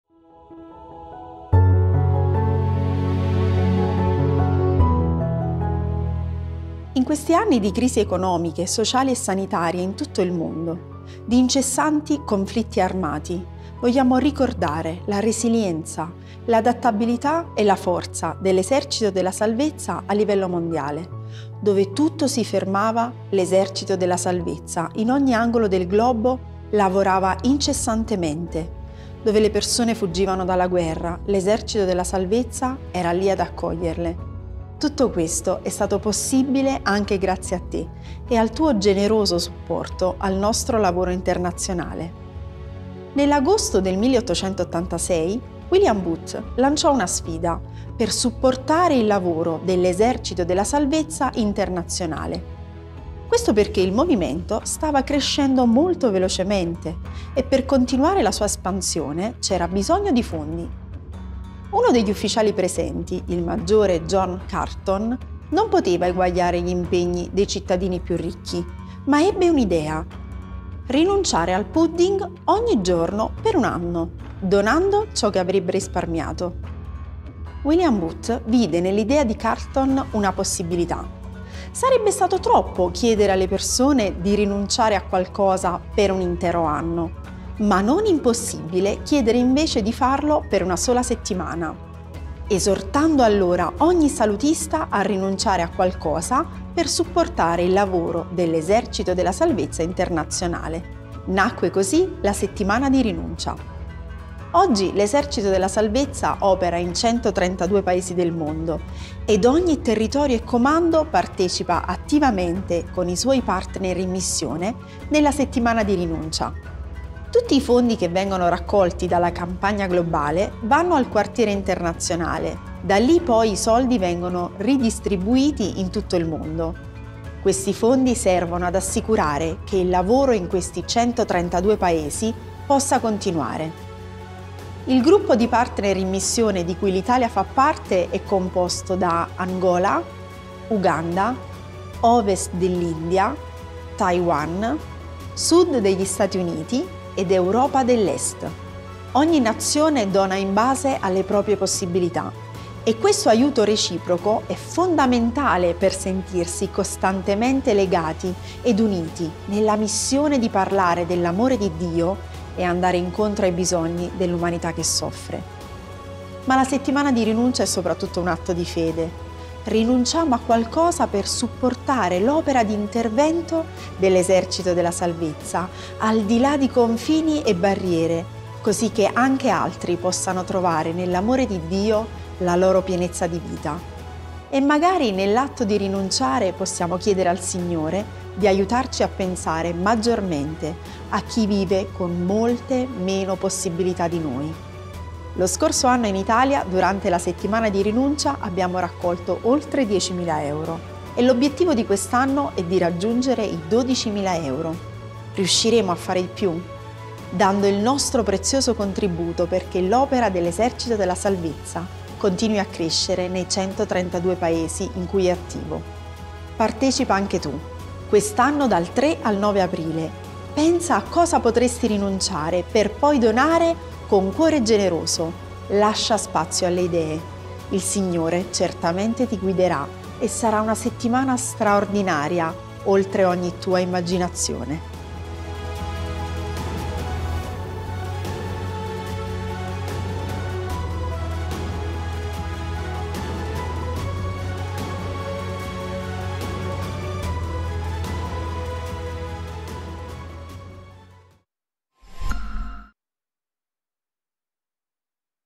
Settimana di Rinuncia 2022
In questi anni di crisi economiche, sociali e sanitarie in tutto il mondo; di incessanti conflitti armati vogliamo ricordare la resilienza, l’adattabilità e la forza dell’Esercito della Salvezza a livello mondiale. Dove tutto si fermava, l’Esercito della Salvezza in ogni angolo del globo lavorava incessantemente, dove le persone fuggivano dalla guerra l’Esercito della Salvezza era lì ad accoglierle.
Tutto questo è stato possibile anche grazie a te e al tuo generoso supporto al nostro lavoro internazionale.

Partecipa anche quest'anno alla Settimana di Rinuncia dal 3 al 9 Aprile! Puoi donare presso il tuo Corpo oppure attraverso bonifico:
IT 67 S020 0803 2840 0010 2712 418  - causale: "Settimana di Rinuncia 2022"